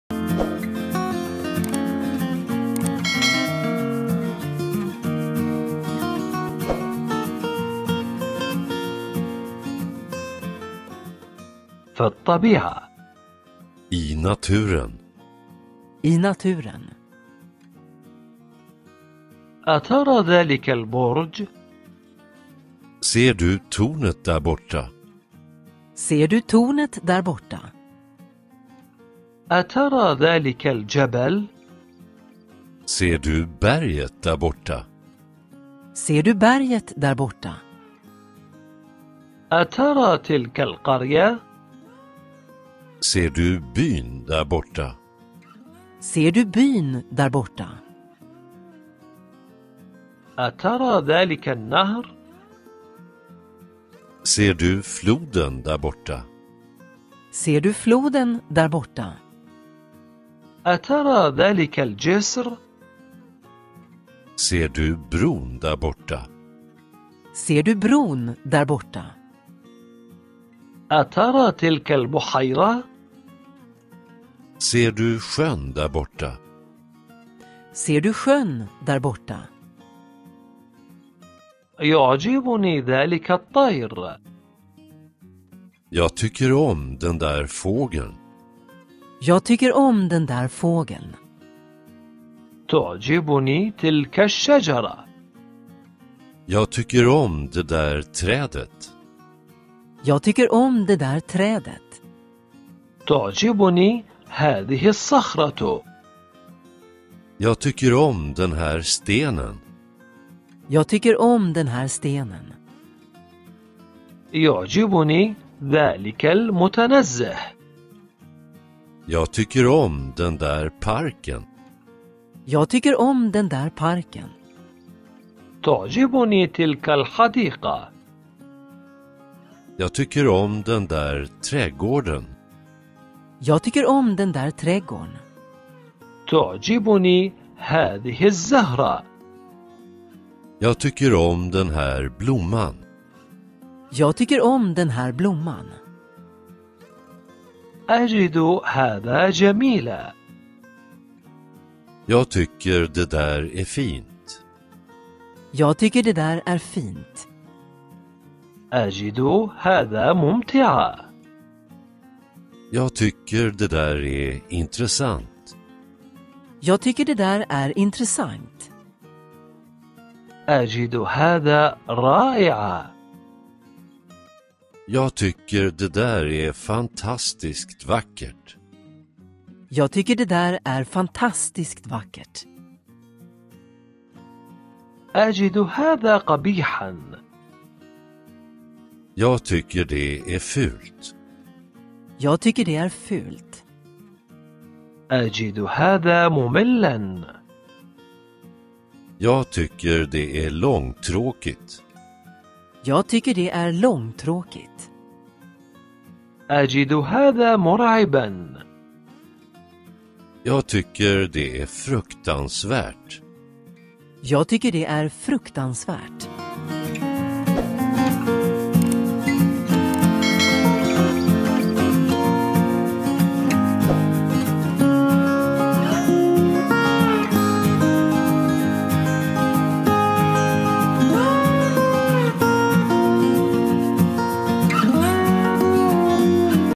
Lär dig svenska på 4 minuter - förbättra talfärdigheterna /I naturen/ تعلم اللغة السويدية
Om du oroar dig för att bli dömd för dina försök att tala på ett främmande språk och den stressen håller dig tillbaka, fråga dig själv det här:

“Hur känner jag när någon som inte talar mitt modersmål perfekt talar till mig? Dömer jag dem negativt eller är jag glad för att de anstränger sig? ”

Ni kan lära själv språk med det viktigte sätt.
Det snabbaste sättet att bli flytande.
Vi lär gärna er mer om det!

إذا كنت قلقًا بشأن الحكم عليك بسبب محاولاتك للتحدث بلغة أجنبية وهذا الضغط يعيقك ، اسأل نفسك هذا:

"كيف أشعر عندما يتحدث إلي شخص لا يتحدث لغتي الأصلية بشكل مثالي؟ هل أحكم عليهم سلبًا أم يسعدني أنهم يبذلون جهدًا؟ "

يمكنك تعلم اللغة بنفسك بطريقة مهمة.
أسرع طريقة للحصول على السوائل.
يسعدنا أن نعلمك المزيد عنه!